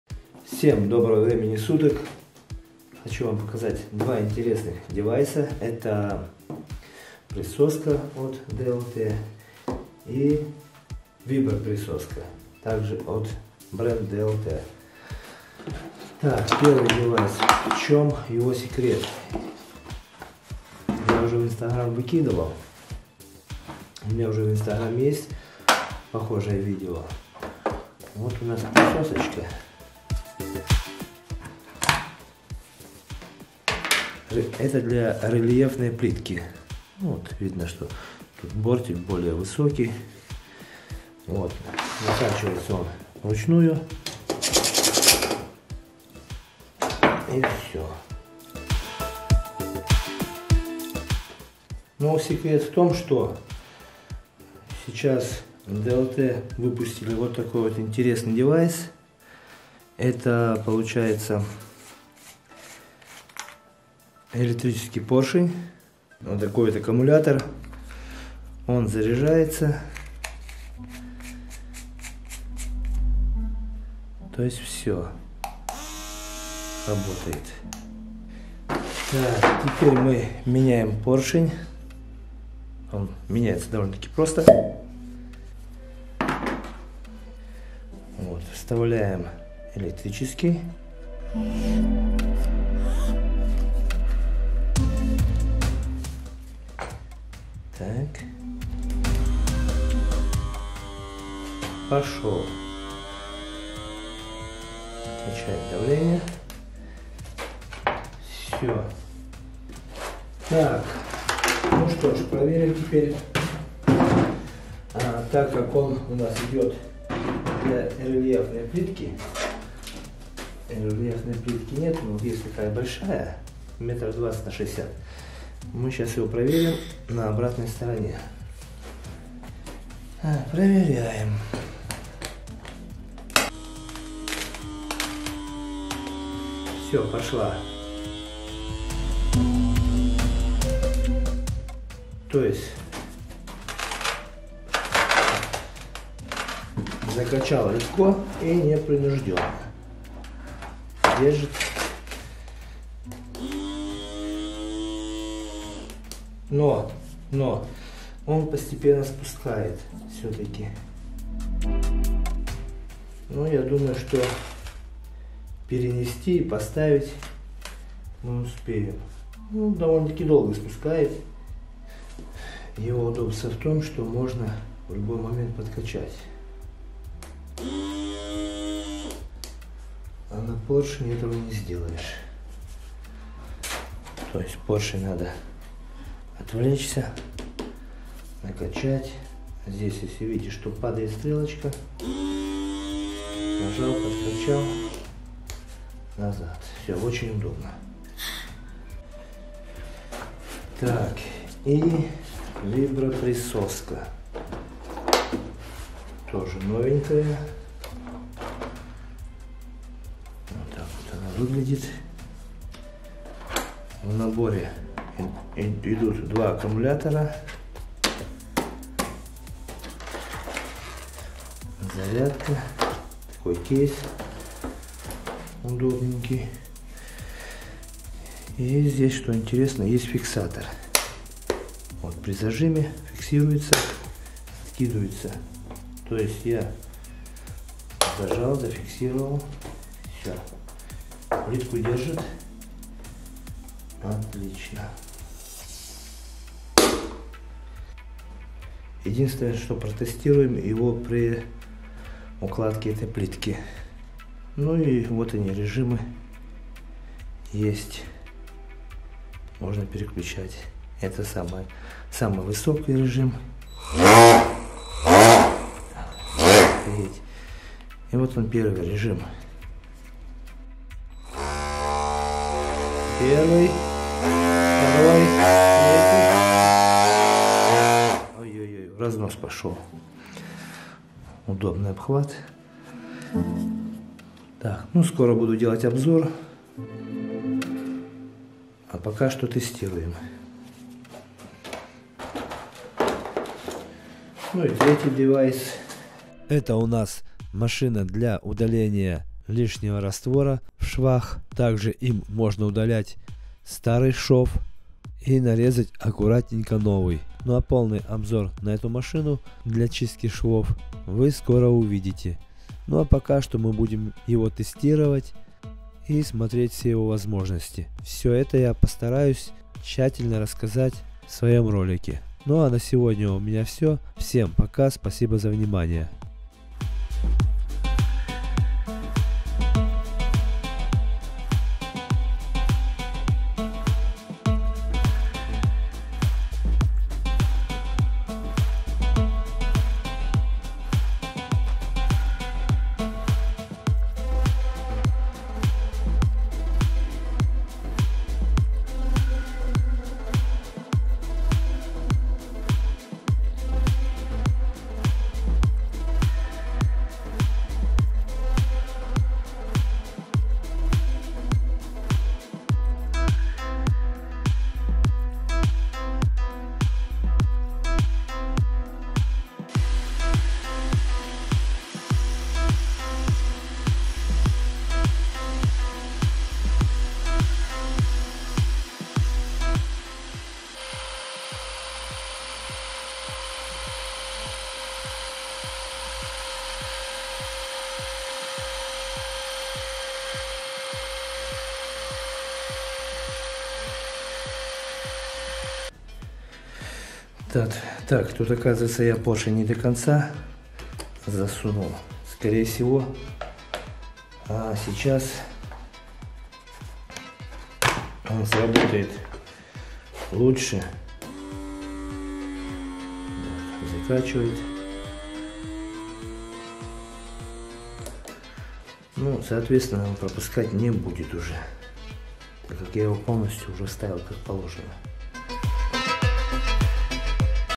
Новый девайс инструментов для плиточных работ.
ЭЛЕКТРИЧЕСКИЙ ПОРШЕНЬ (НАСОС) ДЛЯ ПРИСОСКИ ВИБРОПРИСОСКА ДЛЯ УКЛАДКИ ПЛИТКИ DLT VIBRO MAX